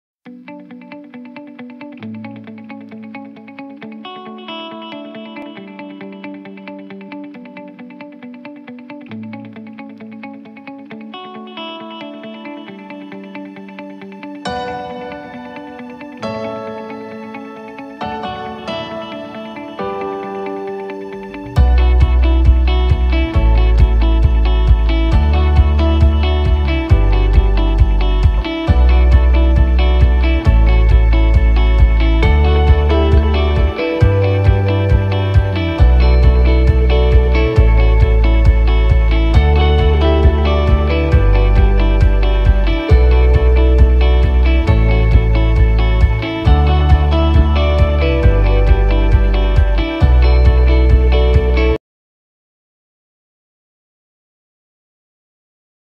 Filhote De Pitbull Com Boxer 2 Mêses (Resultado) 22/10/2020
a mãe é  pitbull terrier    o pai  Boxer